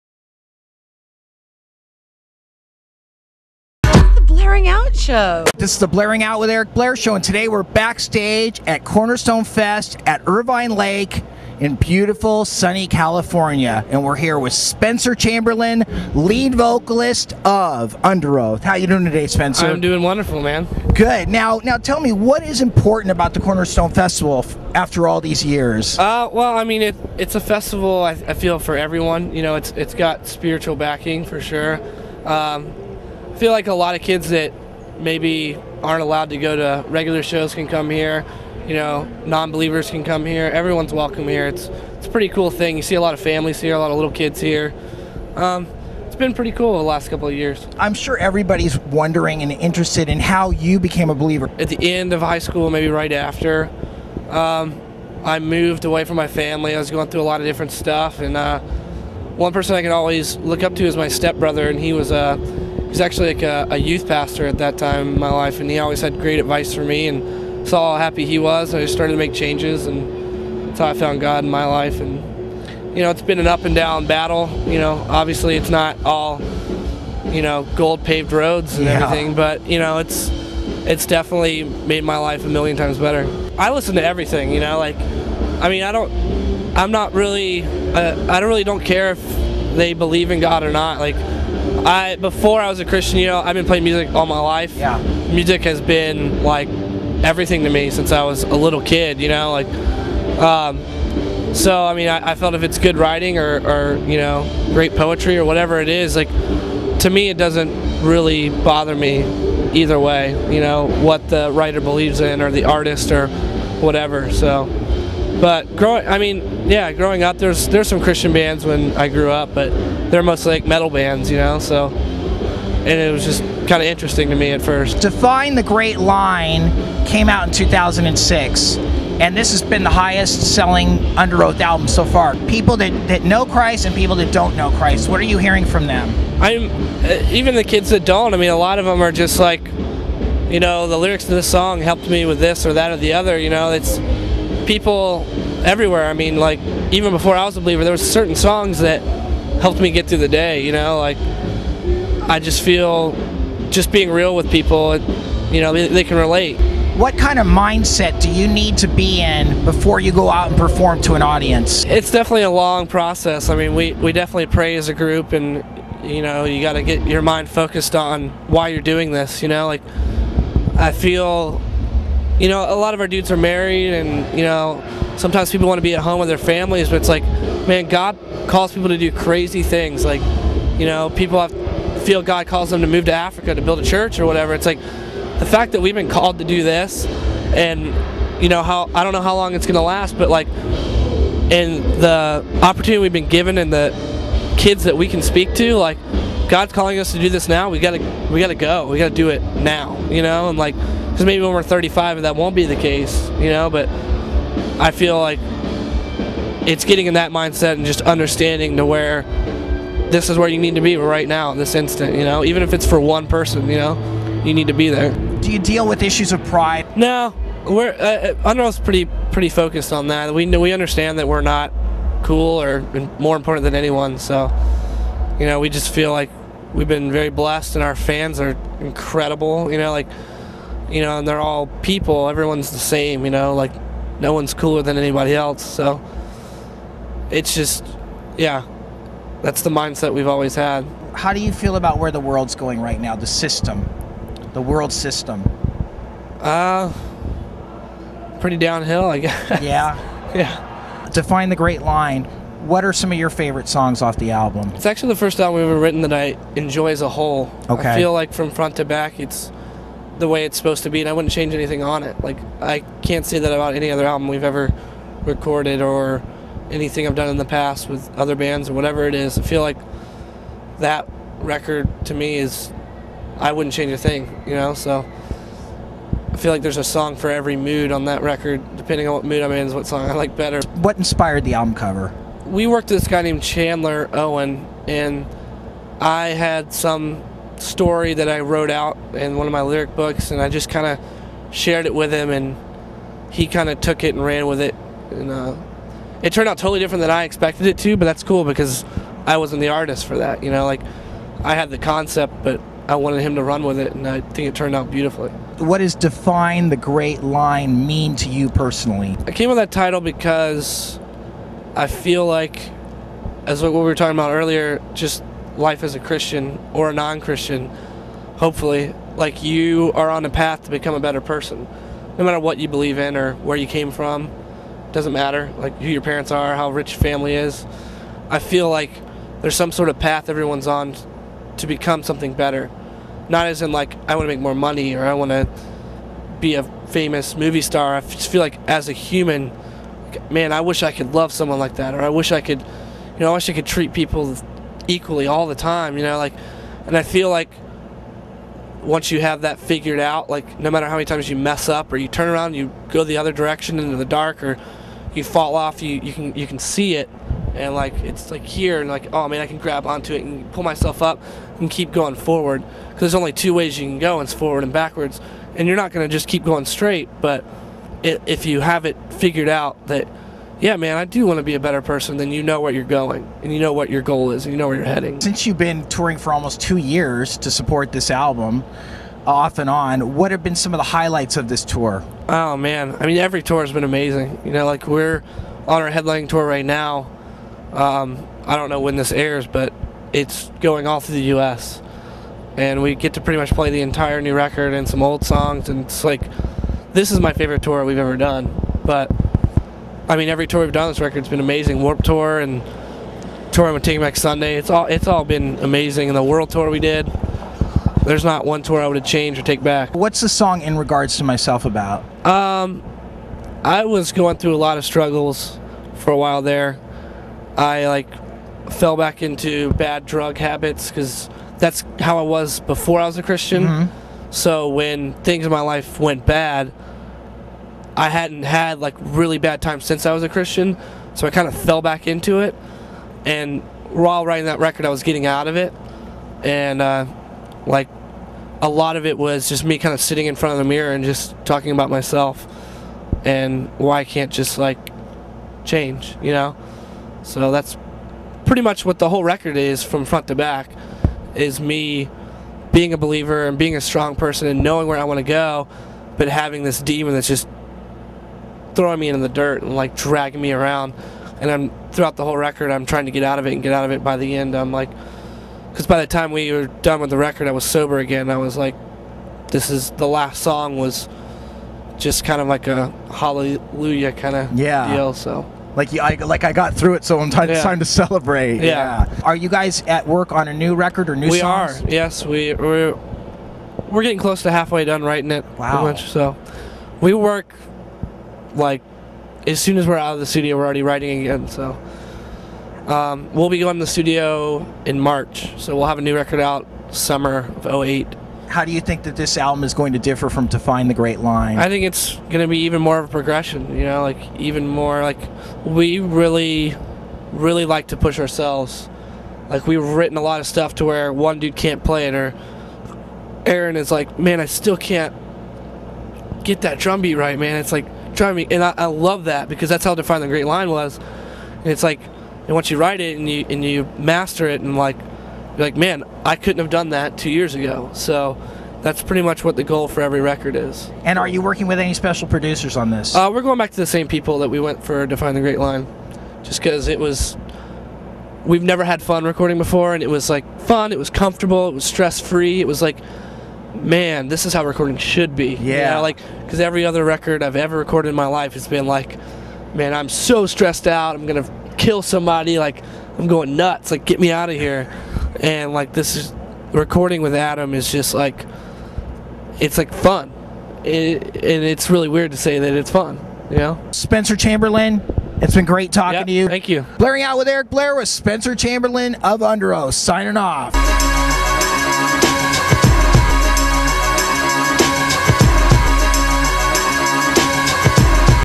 Spencer of UNDEROATH talks with ERIC BLAIR 07
THIS WAS SHOT BACKSTAGE @ THE CORNERSTONE FEST 2007 ERIC BLAIR talks with
Spencer Chamberlain
of UNDEROATH about his life in musc